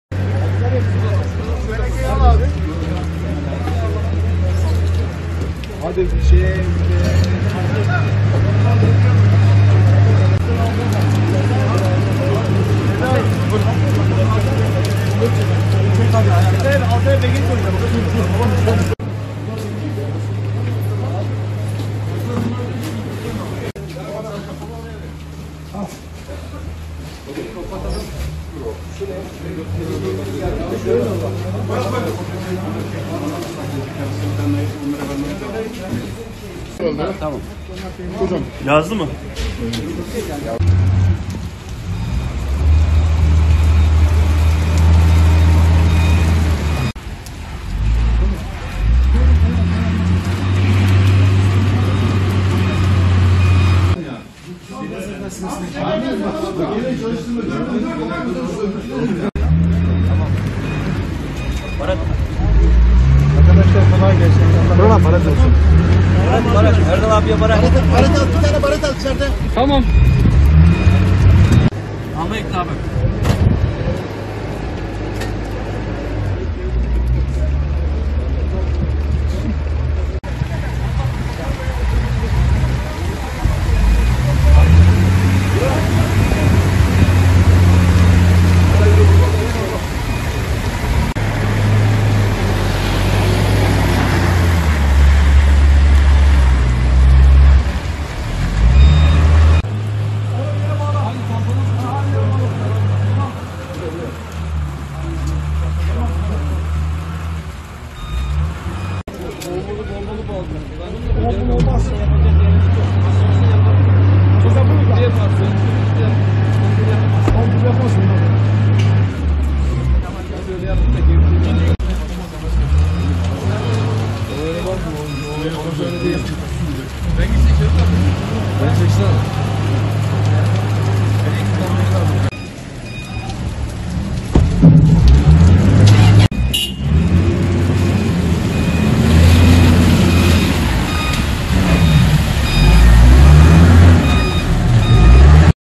Madenciler deprem bölgesine hareket etti
Türkiye Taşkömürü Kurumu tahlisiye personeli deprem bölgesine hareket etti.